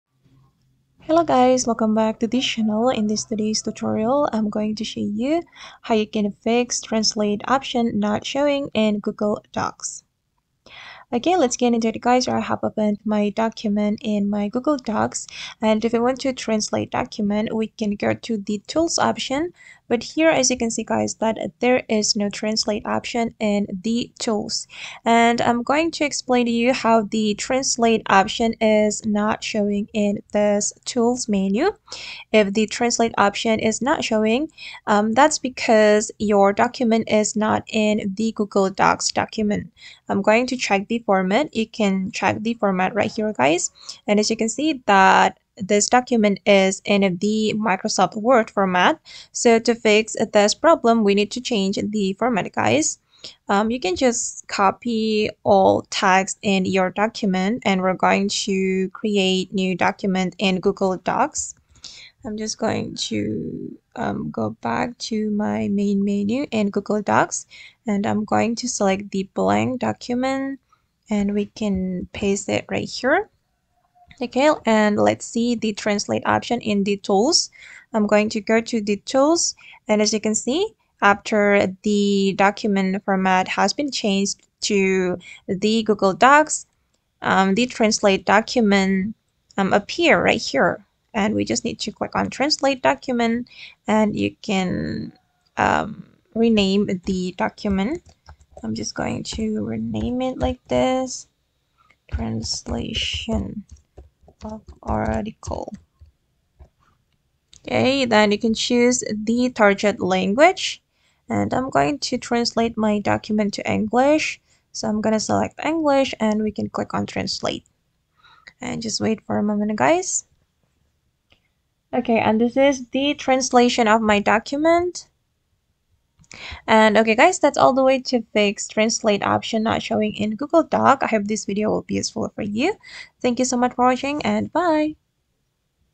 SOLVED! How to Fix Translate Option Not Showing in Google Docs
Can’t find the Translate option in Google Docs? You’re not alone, and I’ve got the solution! In this video, I’ll walk you through the steps to fix the missing Translate feature in Google Docs so you can easily translate your documents again. No more searching for workarounds – this quick fix will get you back on track!